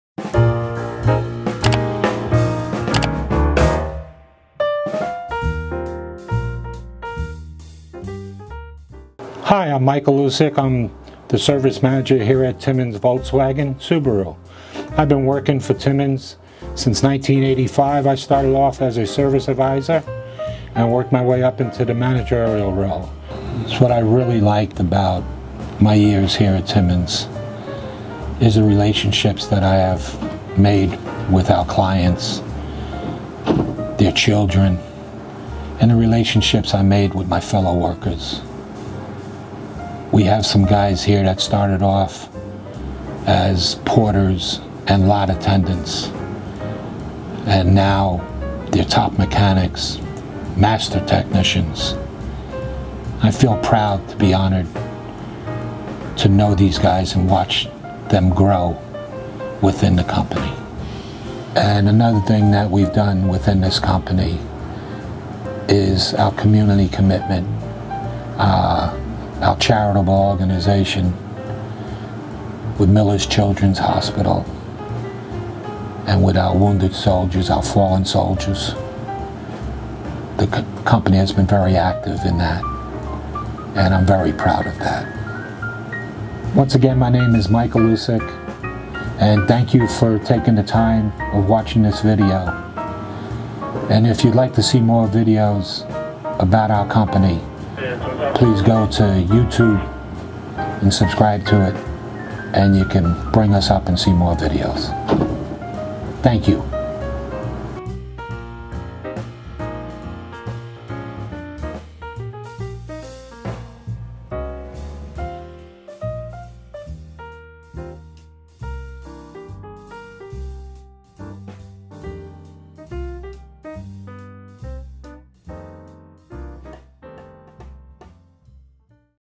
Employee Spotlight - Mike Alusick
Mike Alusick has seen many faces come and go working at Timmons of Long Beach for nearly 35 years! Mike recounts the many times he has helped dealership guests during the times they needed it most as a service adviser and tells us the importance of building relations with those guests and his fellow employees. Through the Timmons of Long Beach Employee Spotlight program, team members are given a chance to speak about the finer points in regards to their careers thus far at the dealership.